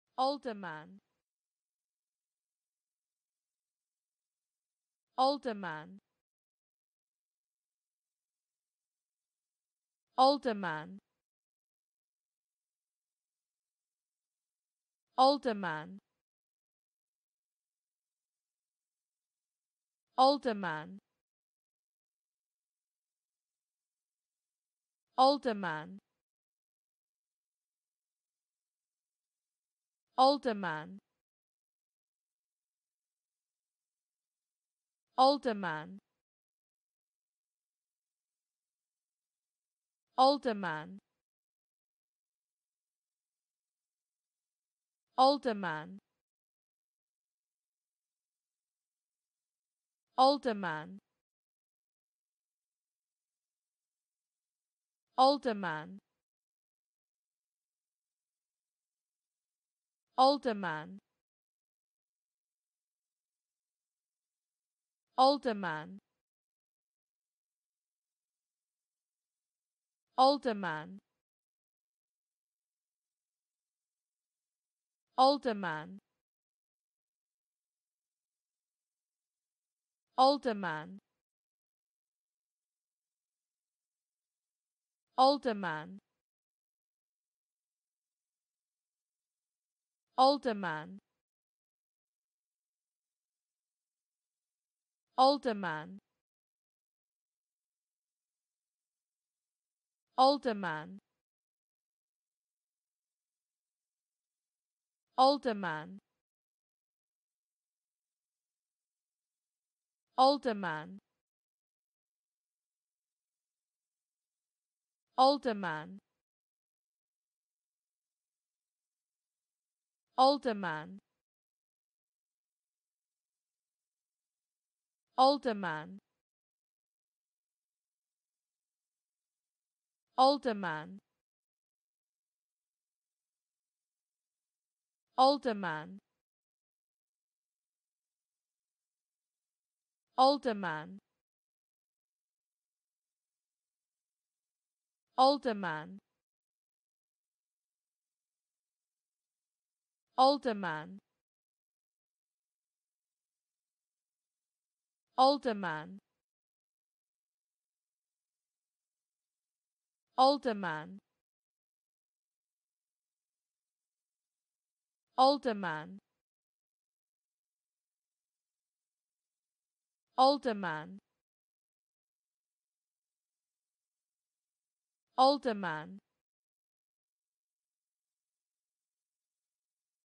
alderman: How to pronounce alderman in english (correct!).Start with A. Learn from me.
Follow me!Learn from me how to pronounce the word alderman in english.This video shows you how to pronounce the word alderman in english.

alderman

n. 市议员；总督；市府参事；高级市政官n. (Alderman)人名；(英)奥尔德曼

/ˈɔːldəmən/

(aldermen)

1.   N-COUNT/N-TITLE

N-COUNT/N-TITLEIn some parts of the United States and Canada, an alderman is a member of the governing body of a city. (在美国或加拿大某些地方的)市政官